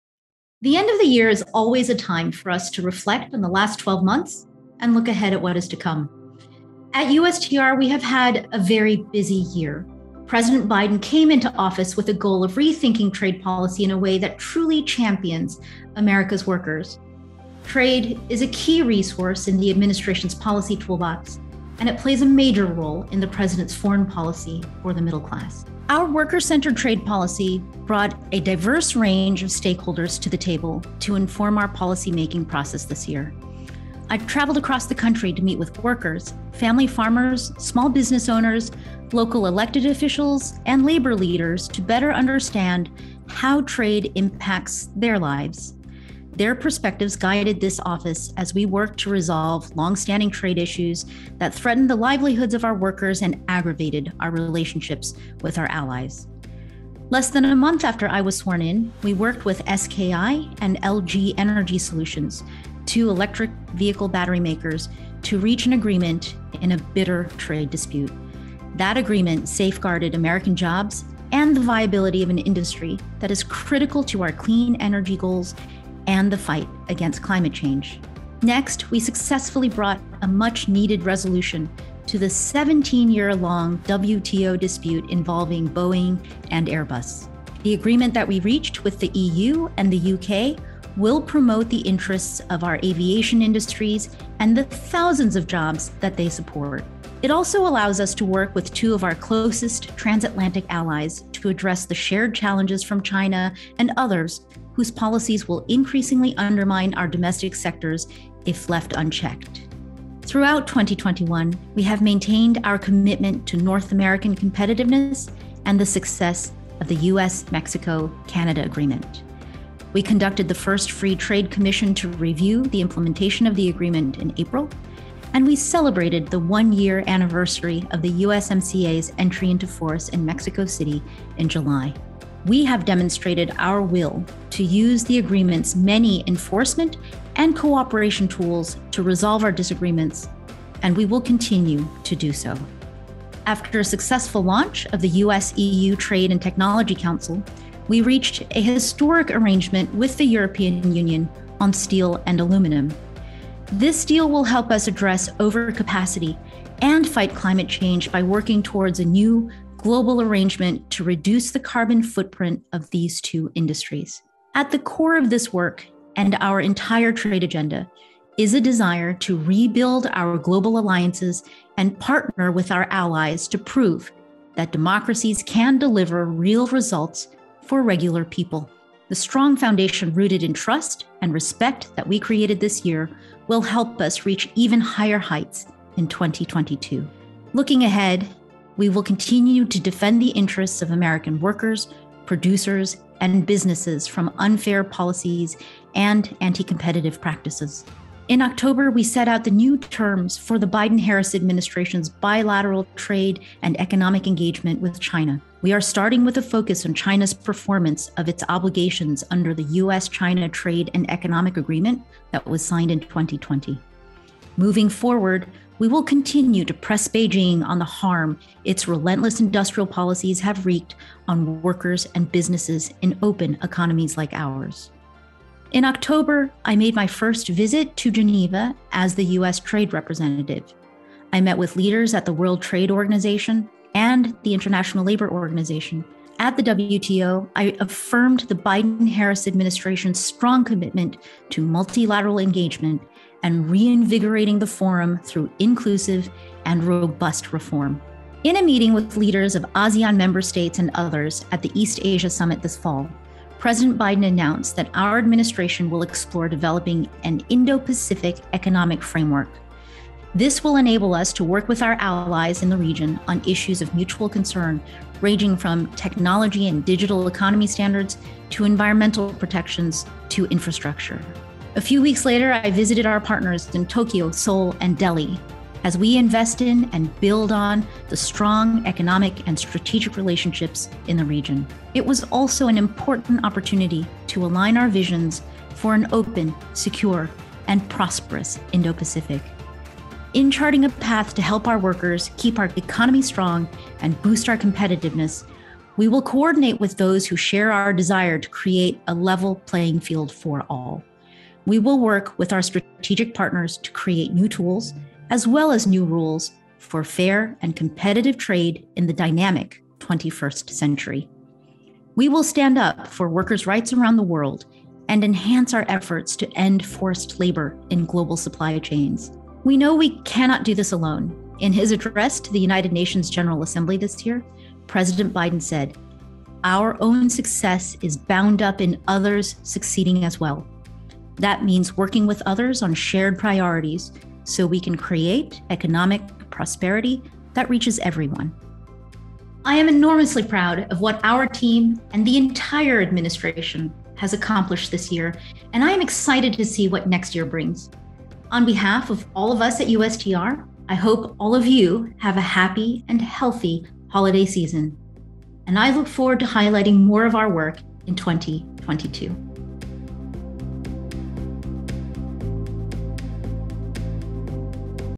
Ambassador Tai 2021 Recap
Watch Ambassador Katherine Tai recap the highlights of 2021 and look forward to 2022.